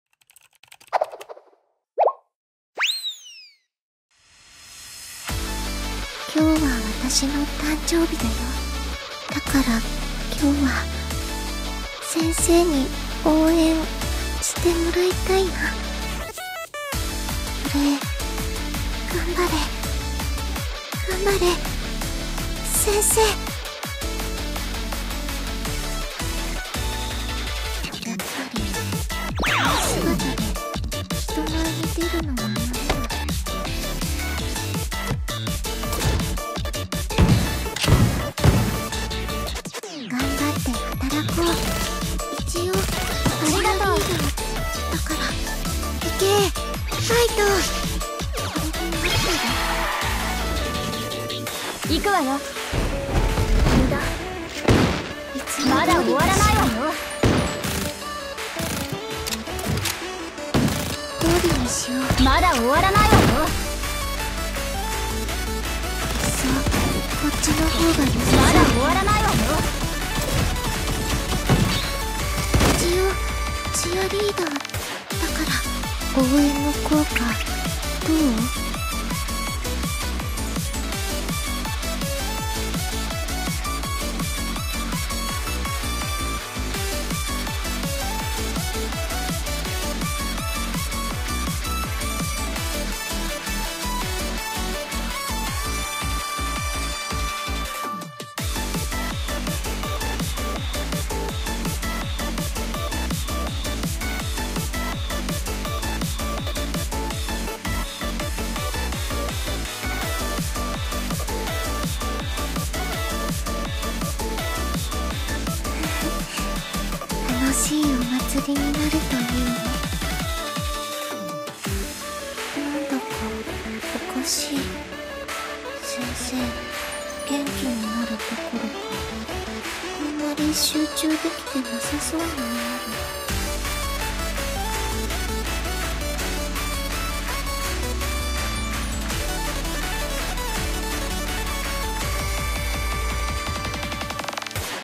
Cute Hibiki (Cheerleader Ver.)
just cute Hibiki in cheerleader costume ⸜( ´ ꒳ ` )⸝

Dunno why I suddenly want to make this, maybe just want to try something very different, or simply bc it's her birthday today.
Anyway, I had fun making this, it's a good experience :V